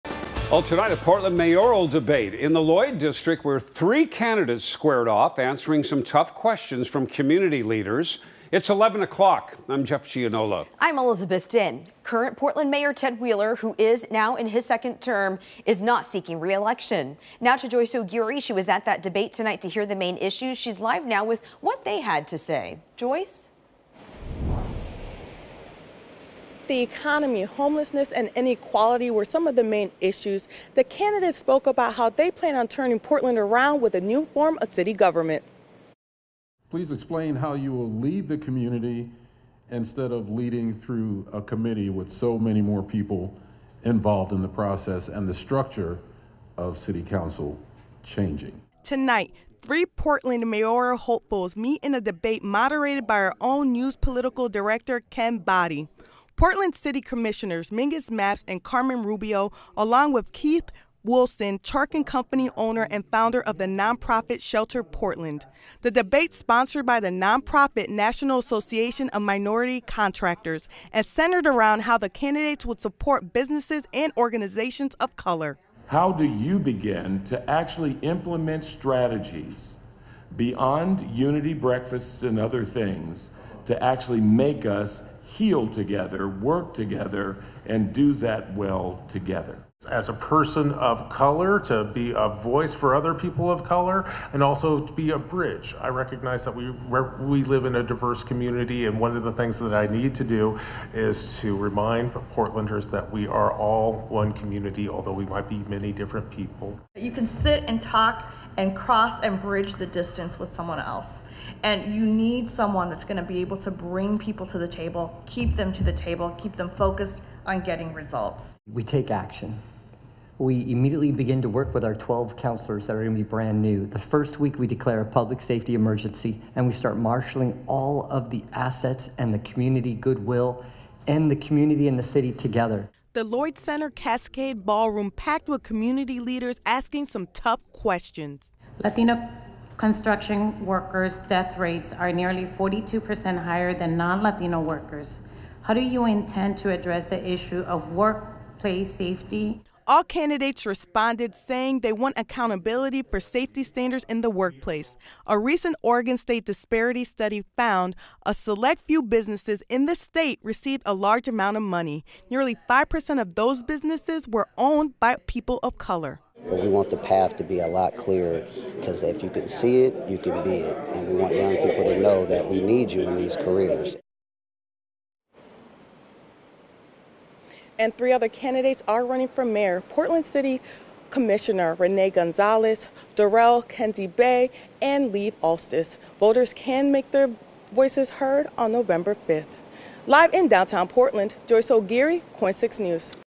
Portland mayoral candidates focus on inequality, uniting city in debate
The economy, homelessness and inequality were some of the main topics discussed during a debate among three people running for Portland mayor Thursday night.